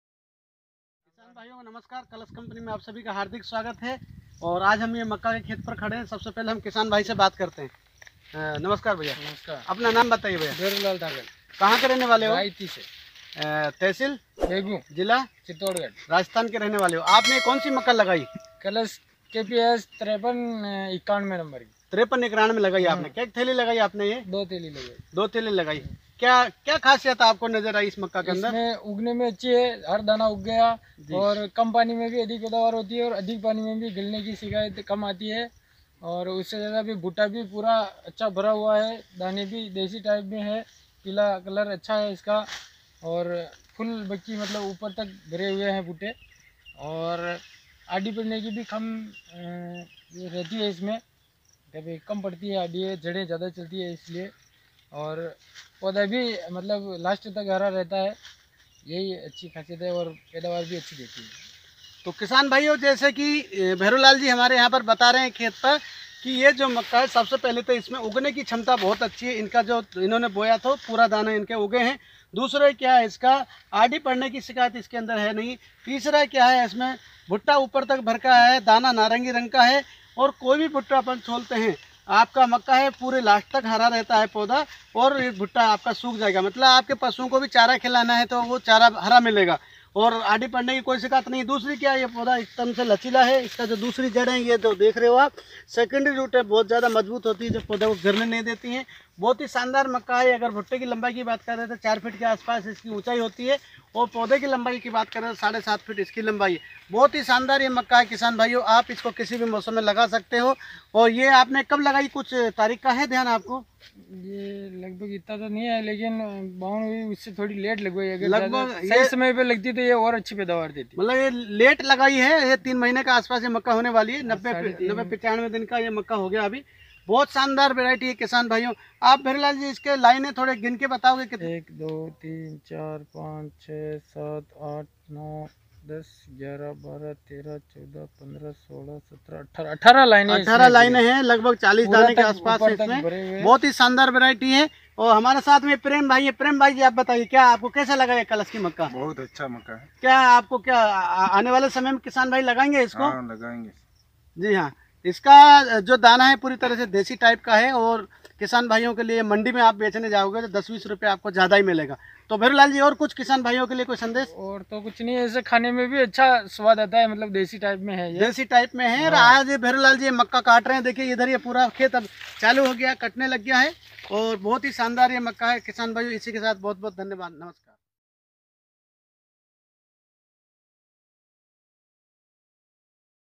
KSP 5391 Rayati Begun
kalash Seeds hybrid maize KSP 5391 very good product high yielding variety attractive orange Colour grain
tip filling